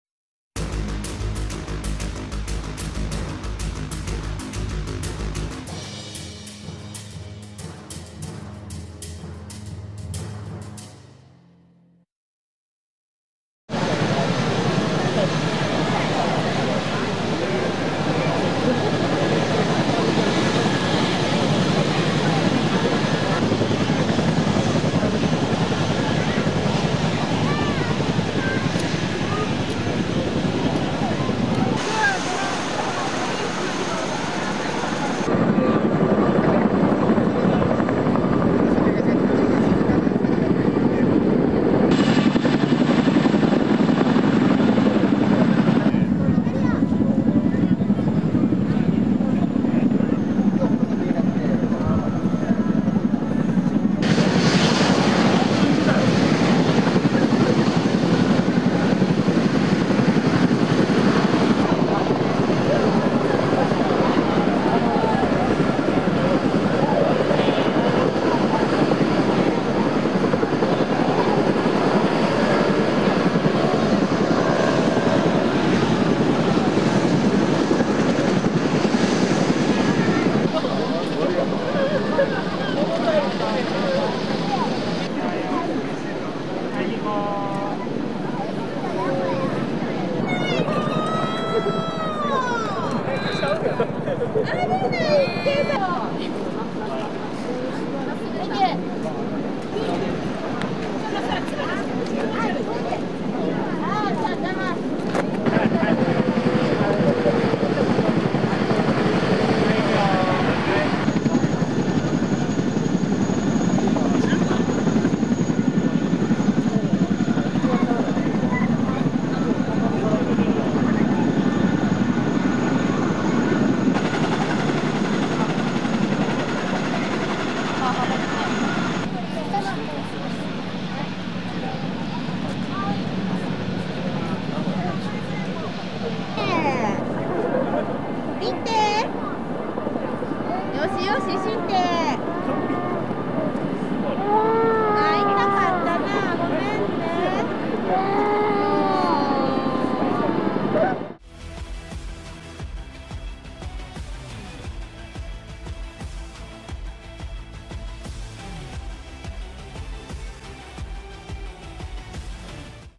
やっちまったぜ！地上展示でまさかのアレ登場【エアーフェスタYAO・八尾駐屯地】
自衛隊動画メインにアップしています。

2023年10月29日に行われた陸上自衛隊・エアーフェスタYAOでの地上展示です。
パンフには特別塗装のアレが見れるかもと書いてましたが、まさかホントにアレだとは…。